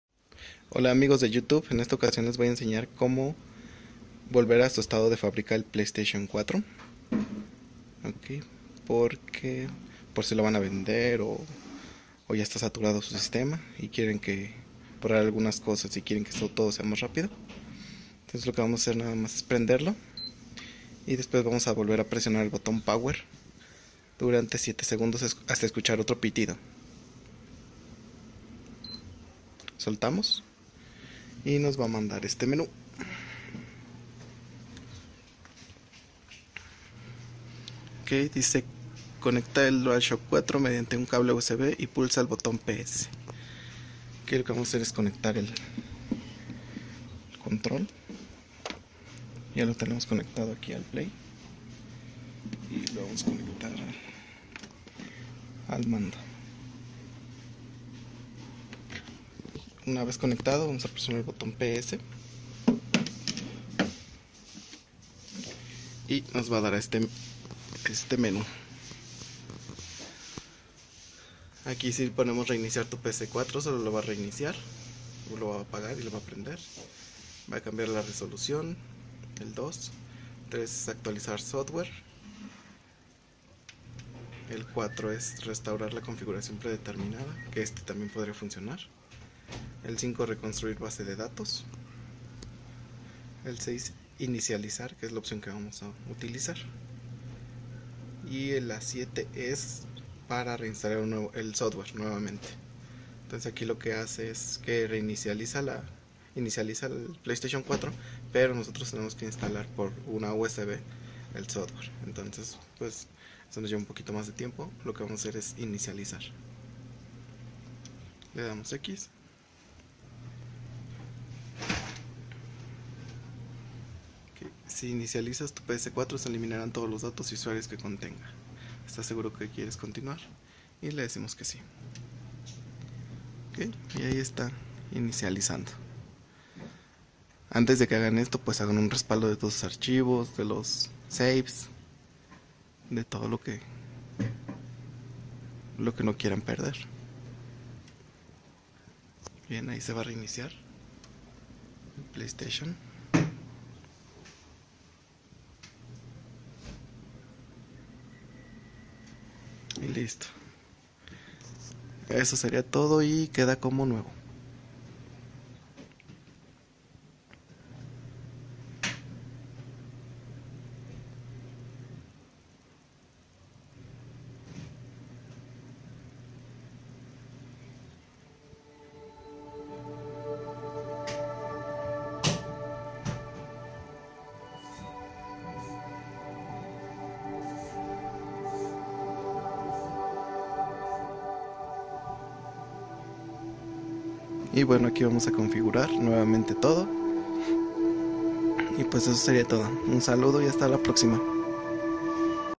Restaurar PlayStation 4 a sus valores de Fábrica
En esta ocasión les voy a mostrar como se restaurar un PlayStation 4 y que quede como si lo hubieran sacado de la caja.